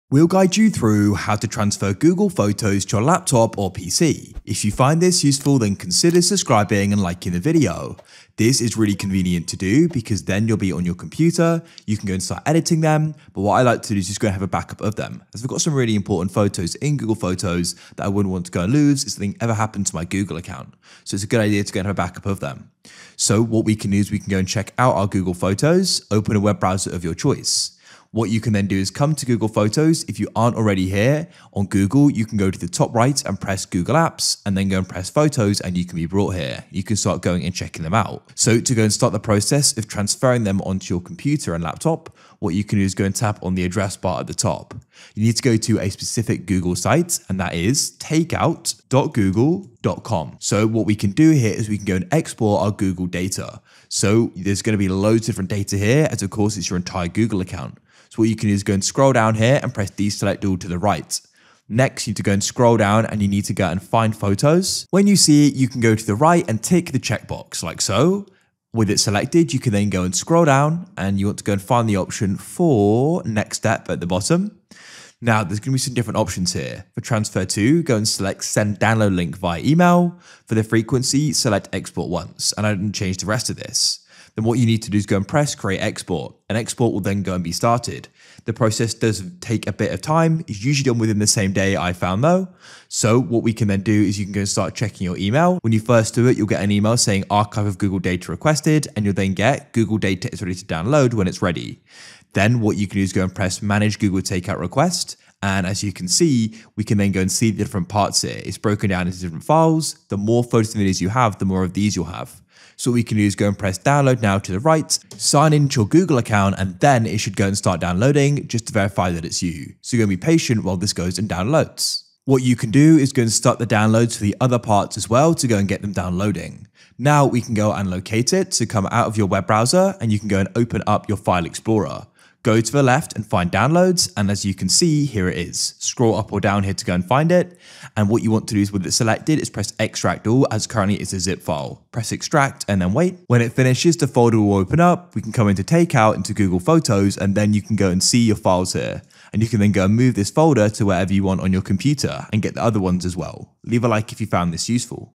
How To Transfer Google Photos To Laptop - Step By Step
Learn how to transfer google photos to laptop in this video.

GuideRealm